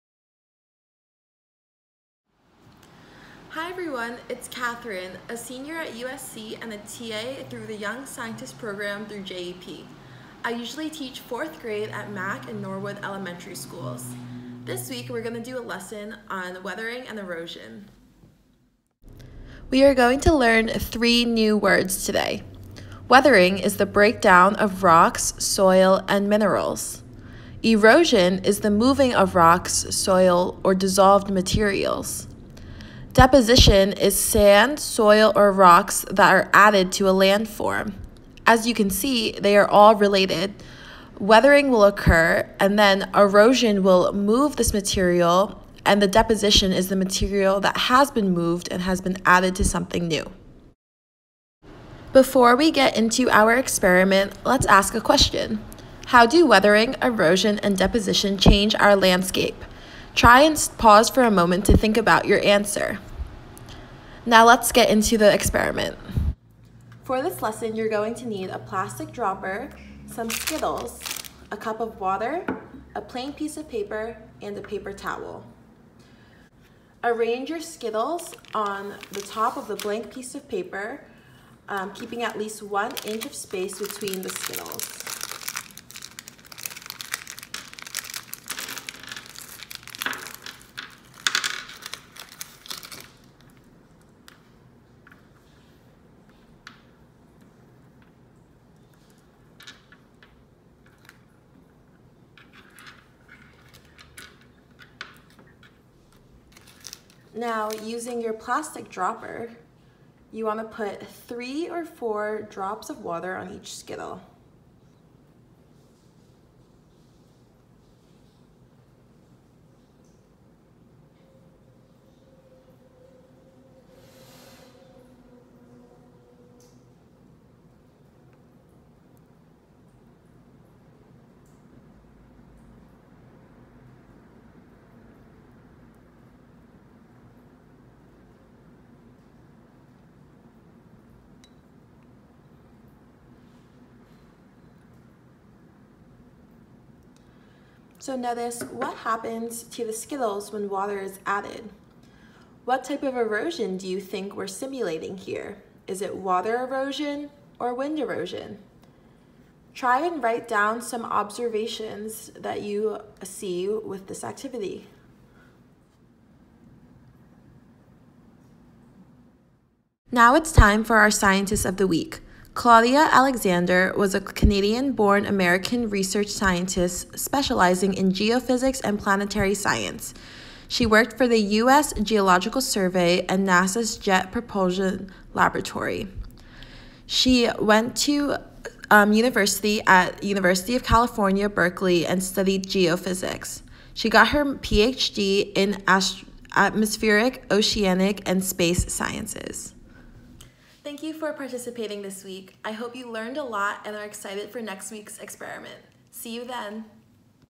USC Young Scientists Program - Fall Semester - Week #3 - 4th Grade - Weathering and Erosion
The USC Young Scientists Program strives to deliver quality science education weekly to a handful of LAUSD elementary schools. Due to the pandemic and prominence of virtual learning, videos like this one are being made to distribute to the teachers. This week, we will be investigating weathering and erosion!

We shall also be featuring non-dominant scientists such as women, and BIPOC individuals that you may or may not have heard of that relate to the science being featured. The scientist being featured here is Claudia Alexander. Claudia Alexander was a Canadian-born American research scientist specializing in geophysics and planetary science. She worked for the United States Geological Survey and NASA's Jet Propulsion Laboratory

Video created by Katherine Shiu

NGSS Essential Standards and Clarifying Objectives: 4​-ESS2-1. Make observations and/or measurements to provide evidence of the effects of weathering or the rate of erosion by water, ice, wind, or vegetation.

This video was funded by the USC Good Neighbors Campaign Grant.